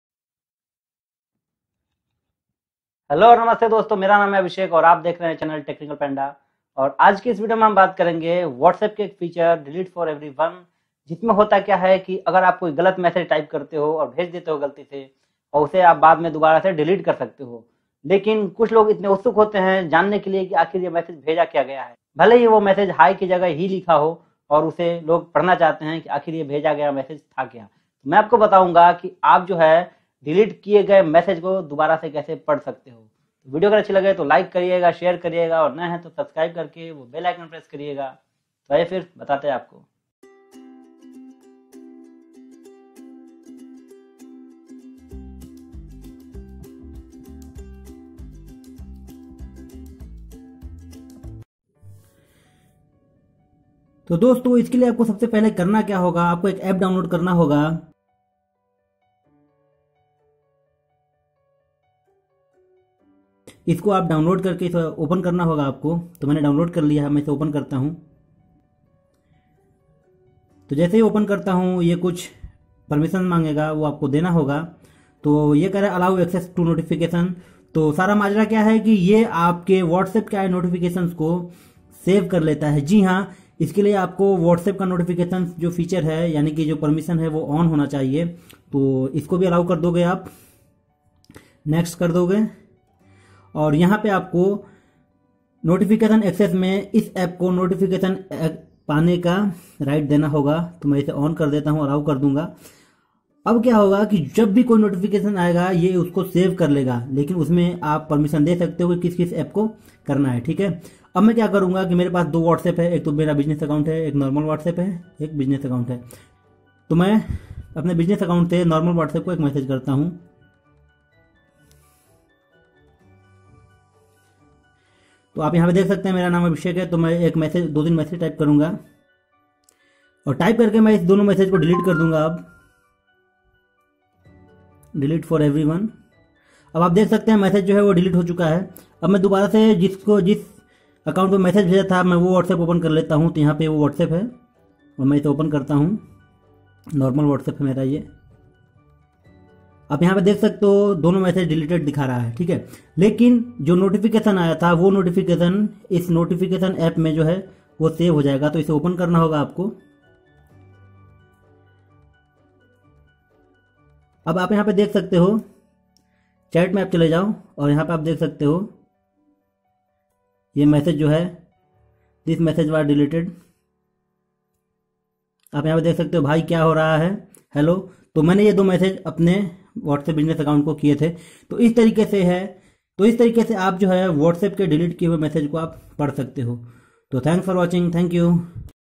whatsapp par delete messages kaise laye
how to read deleted messages on whatsapp.
Dosto aaj ki is video me ham janege ke aap whatsapp par delet for everyone fichar ke madad se delet kiye gaye massge ko kaise padh sakte ho.